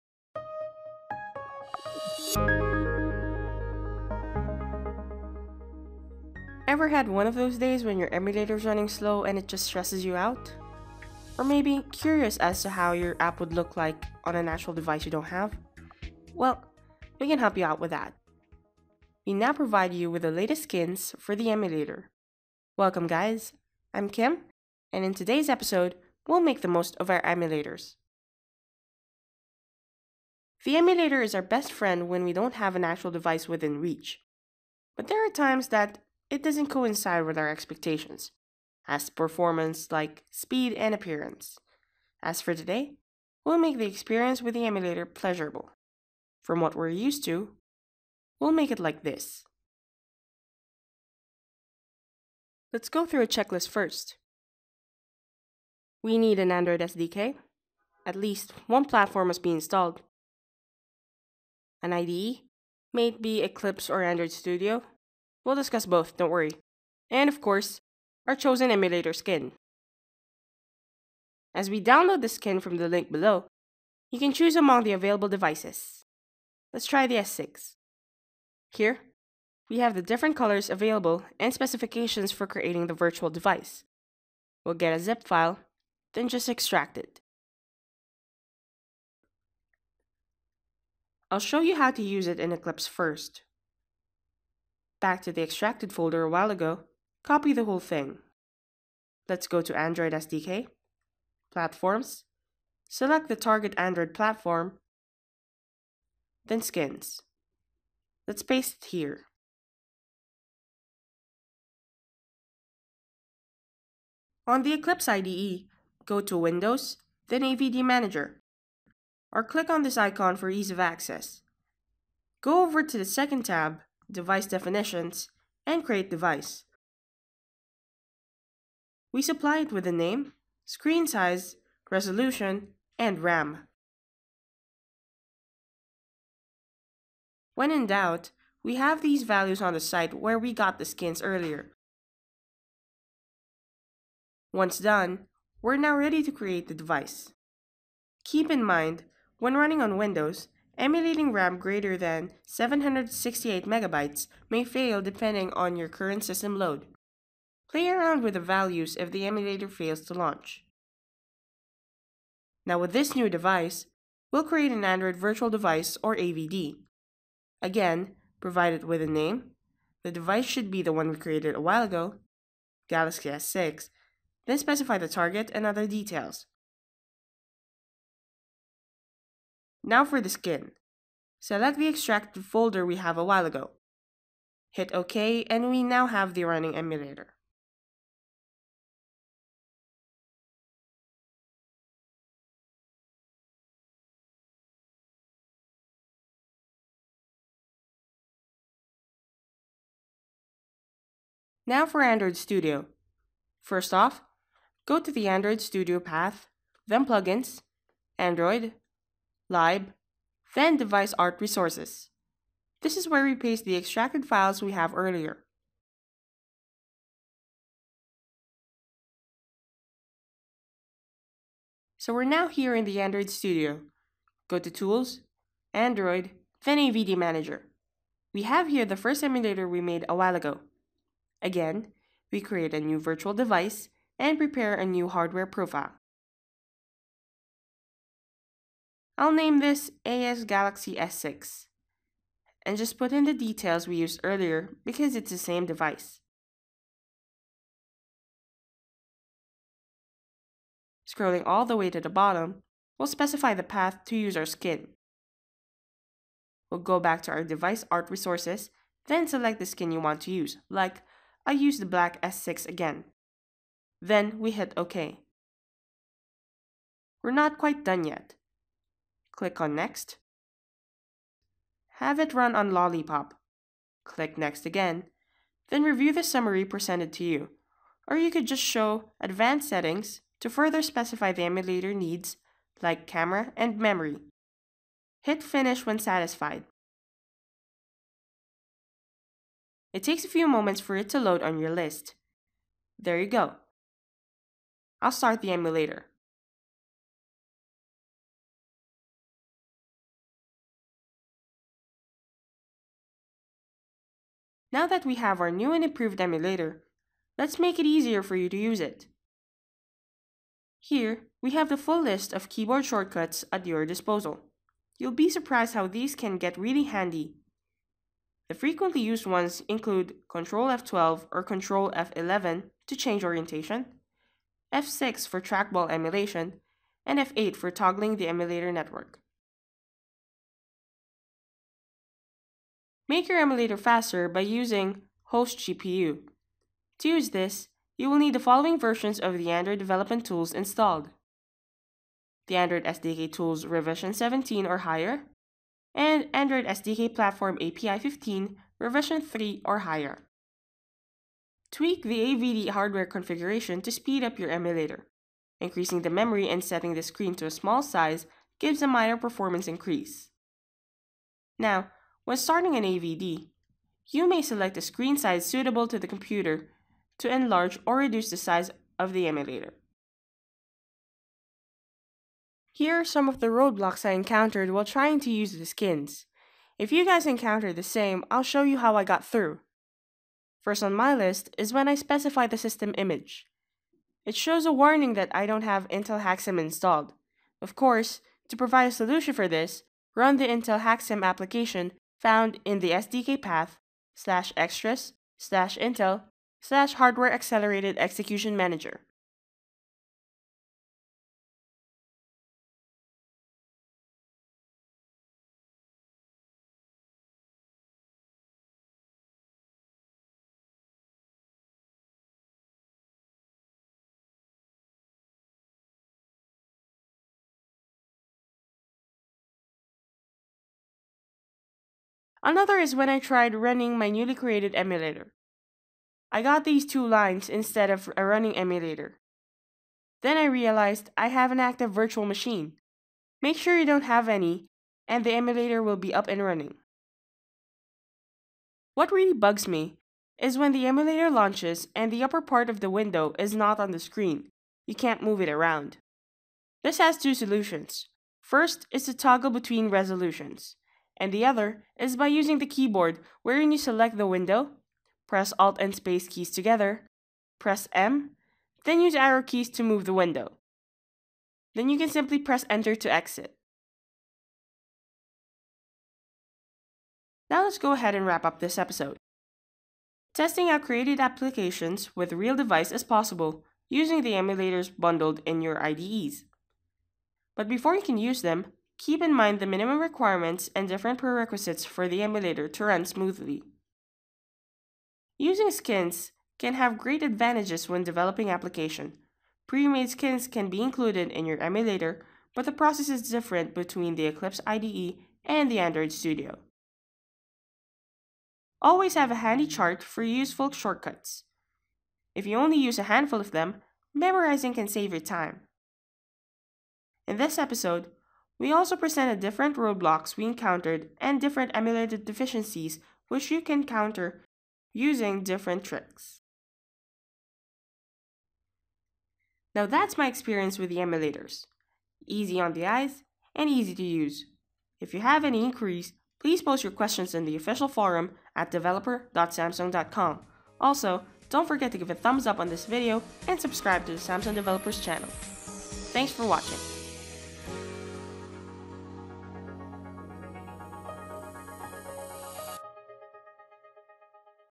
[Tutorial] Using Emulator Skins - Test Your App on Galaxy S6 Skin
Testing your app on an emulator that comes with the IDE makes app development less troublesome. But it doesn’t quite compare with the actual device as to appearance and performance, until now.

This video shows you how to make use of emulator skins to have the look of the actual device. It also gives you a few tricks up your sleeve to improve the speed and your experience with the emulator.